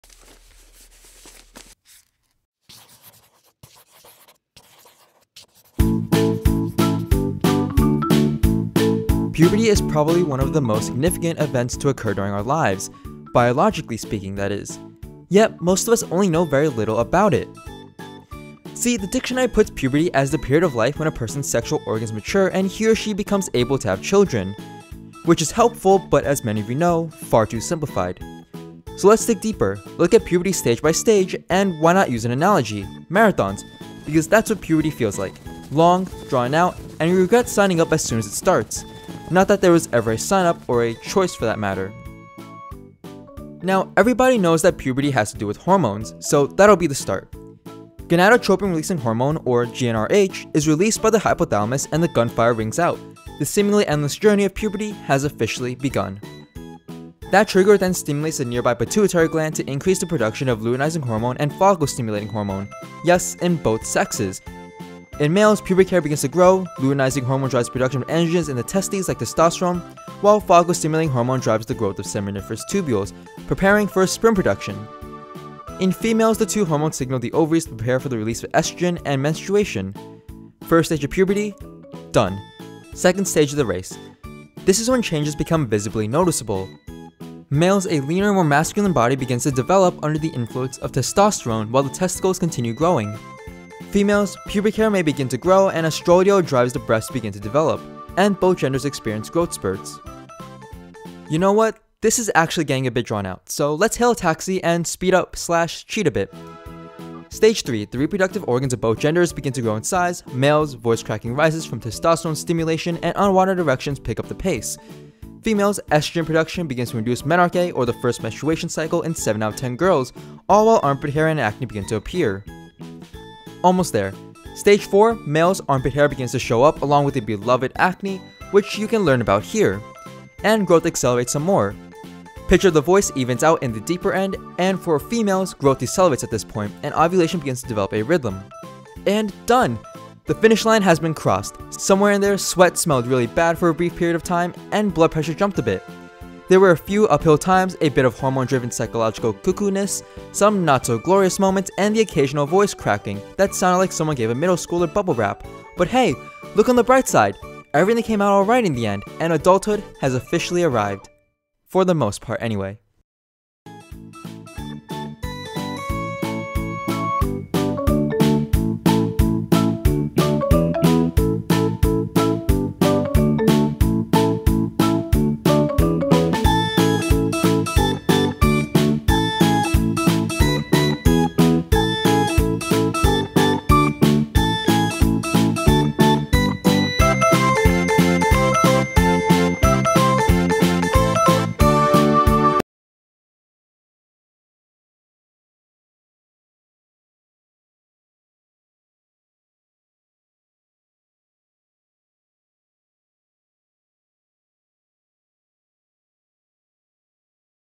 What is Puberty?
Puberty is one of the most significant events to occur during our lives, biologically speaking. Yet, most of us only know very little about it. For instance, did you know that puberty has been divided into stages by developmental biologists, from 1 to 5 (done).

These stages run from growth, to sperm production (males only), menarche/menstruation (females only), and other puberty-induced changes (i.e. voice deepening, breast development, etc.).

Want to learn more? Of course you do! Watch the video and enjoy! :)